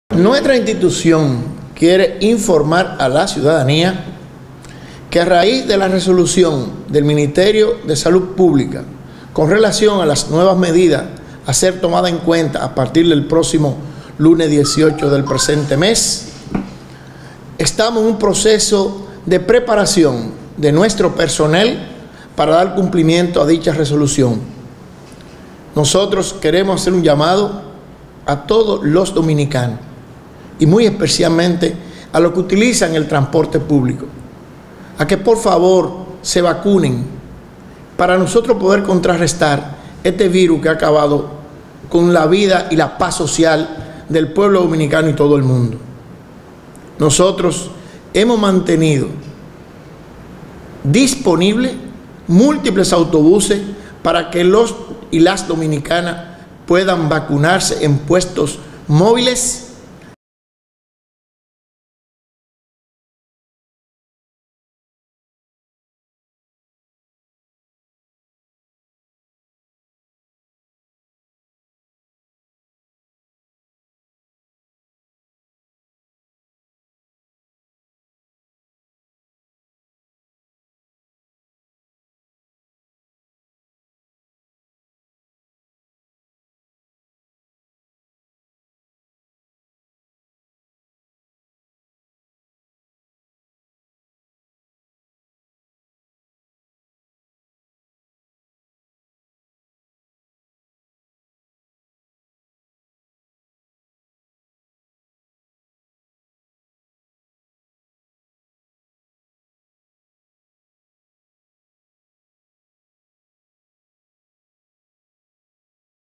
OMSA informa se prepara para asumir nuevas medidas contra el Covid -19
La Oficina Metropolitana de Servicios de Autobuses (OMSA) informa que la entidad de transporte público se encuentra en un proceso de preparación para asumir las nuevas medidas sanitarias, establecidas en la Resolución 048 emitida por el Ministerio de Salud Pública, tras el levantamiento del estado de emergencia dispuesto por el Poder Ejecutivo.

La entrada en vigor de dicha resolución, que dispone que los usuarios deberán presentar tarjeta de vacunación o prueba PCR Covid negativa al abordar cualquier medio de transporte colectivo, será a partir de las 5:00 de la mañana del próximo lunes 18 de octubre.

En ese sentido, el director general de la OMSA, Radhamés González, motiva a la población a vacunarse, para poder dar cumplimiento a las nuevas medidas sanitarias, a fin de contrarrestar las consecuencias negativas del Covid-19.

Explicó que desde la entidad se están tomando las medidas de lugar para dar cumplimiento a la resolución emitida por las autoridades sin que esto afecte la operatividad del servicio de transporte que se le ofrece a la población.

Aclara que hasta el momento la única exigencia para los usuarios de los autobuses de transporte público de la OMSA es el uso obligatorio de mascarilla y mantener el distanciamiento social.